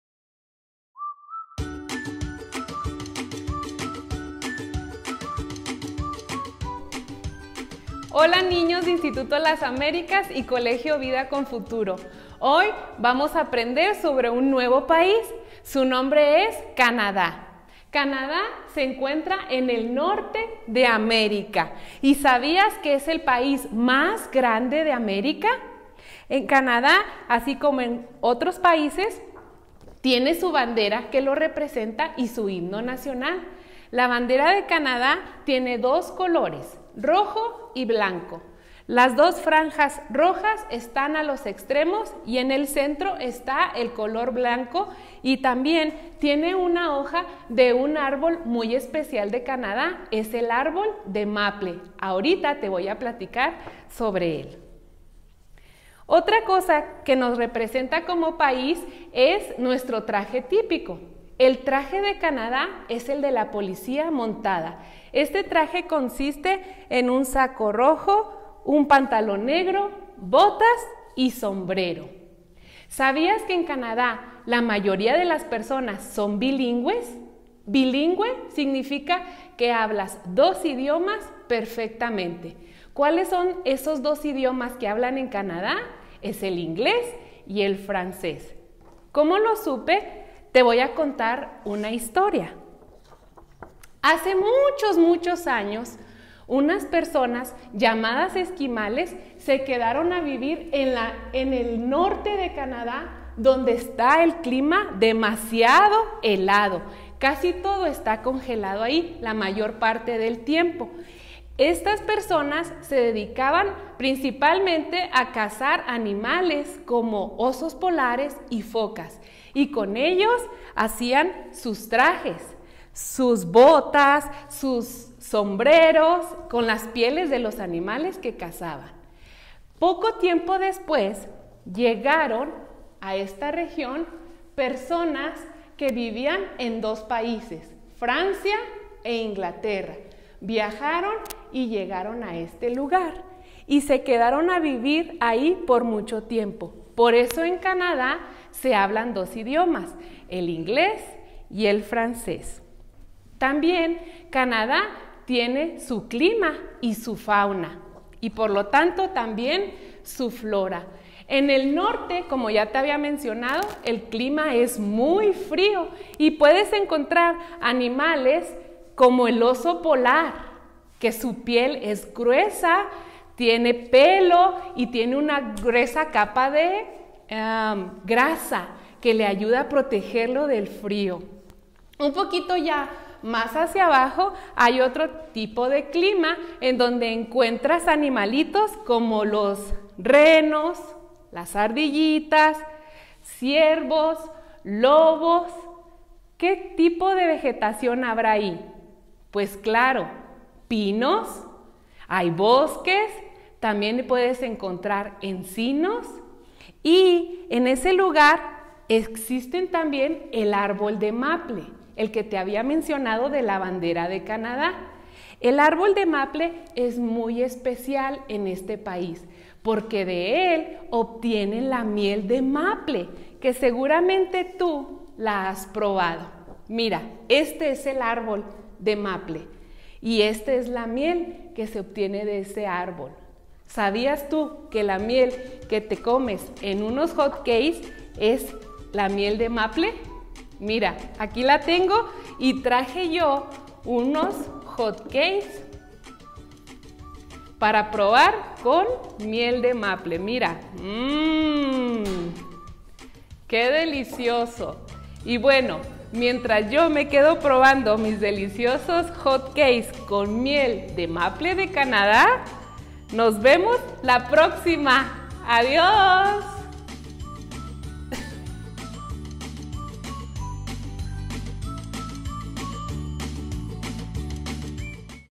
El país de Canadá | ILAP en casa
La maestra Irlanda nos habla acerca de Canadá.